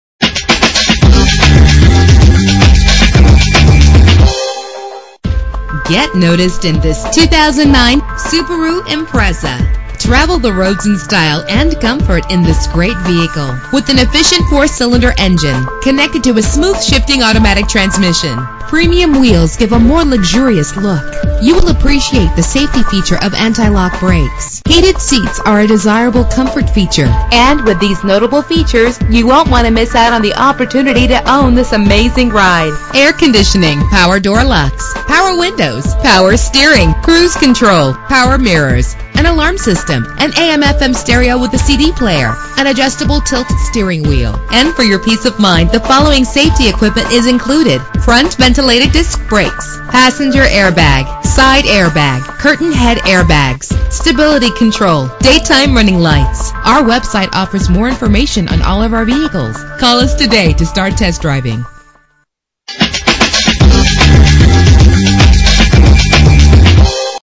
2009 Subaru Impreza - Bountiful UT
Year: 2009
Make: Subaru
Model: Impreza
Trim: Outback Sport
Engine: 2.5 liter H4 16 valve
Transmission: 4-Speed Automatic
Color: Black
Mileage: 34407
Address:
2380 South Hwy 89